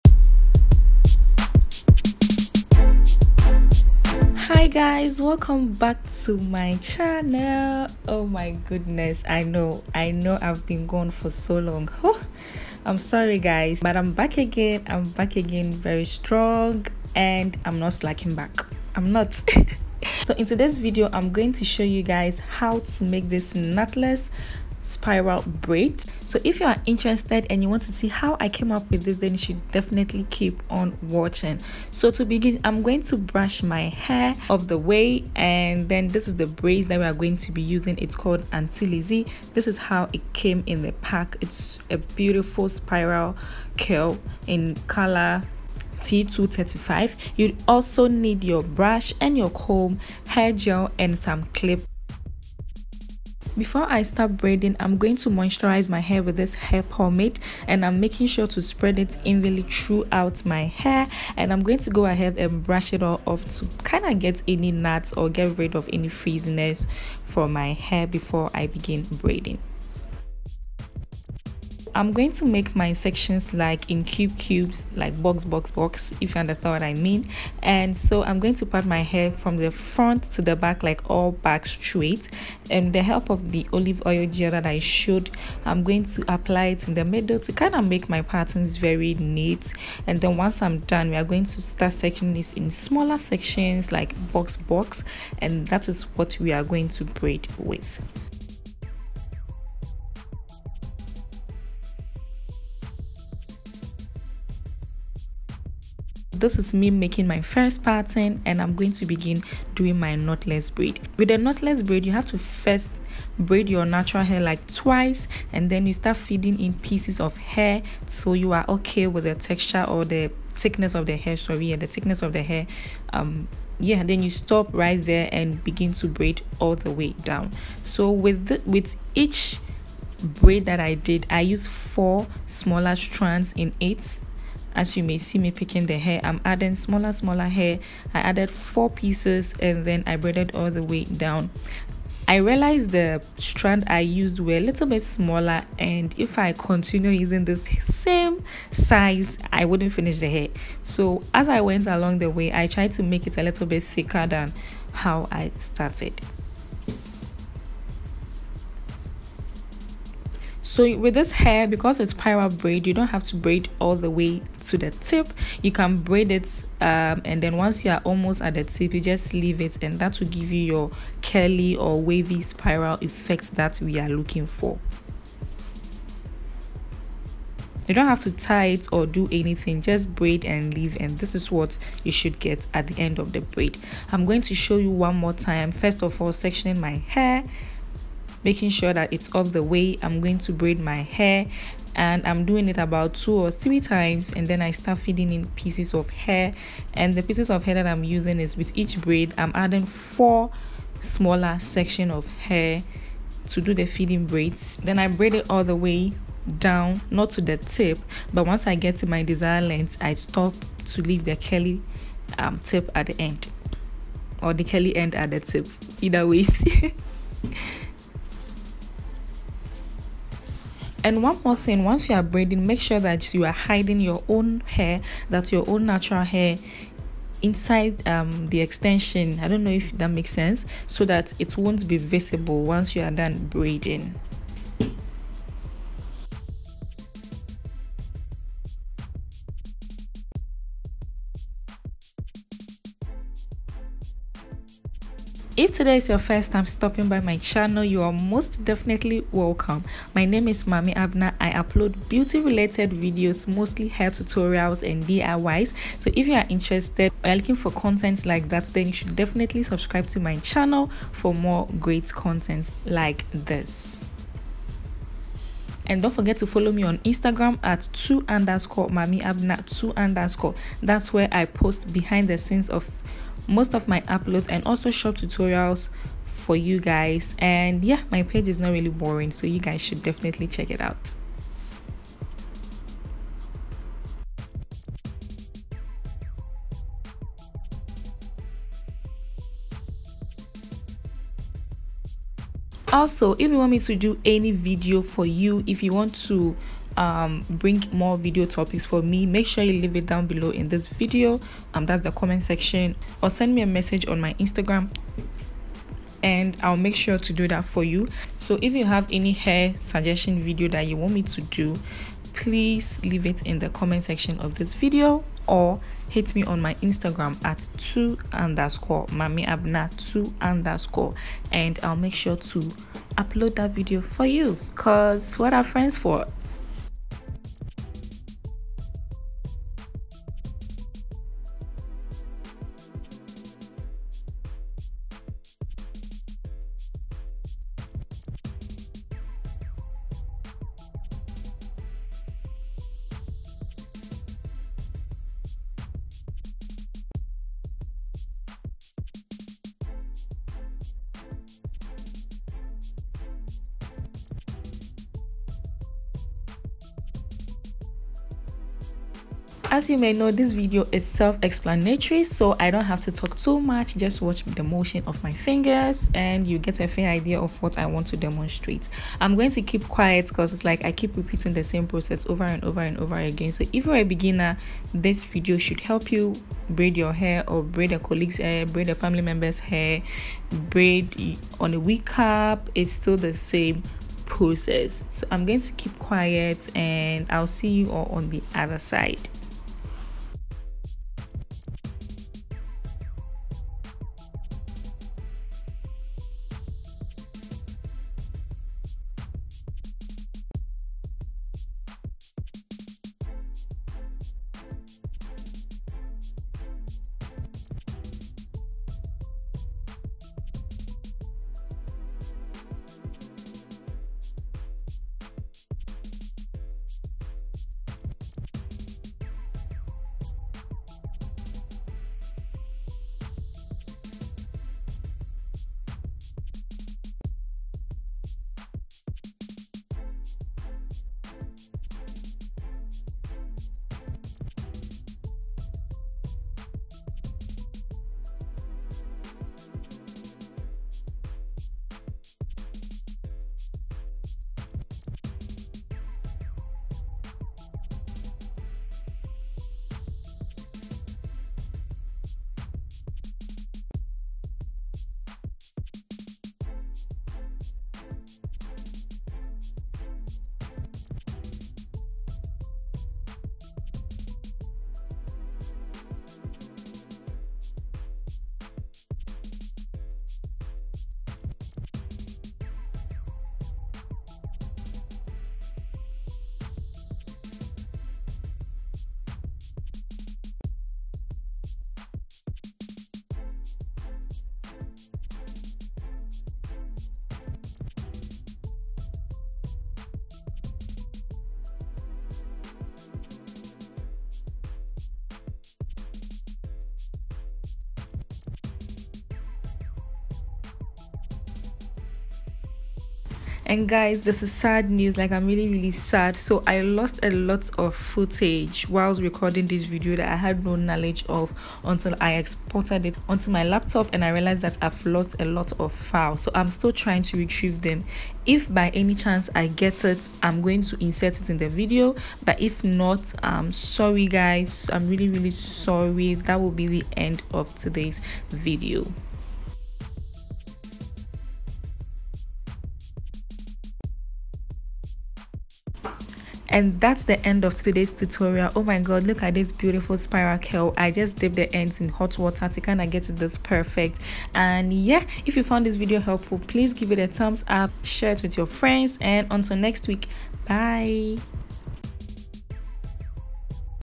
HOW TO: knotless Spiral Braids on Yourself || HOW TO DO YOUR OWN BOX BRAIDS
How to do knotless spiral braids on yourself

In todays video im going to demostrate to you how you can achieve this beautiful spiral braids in no time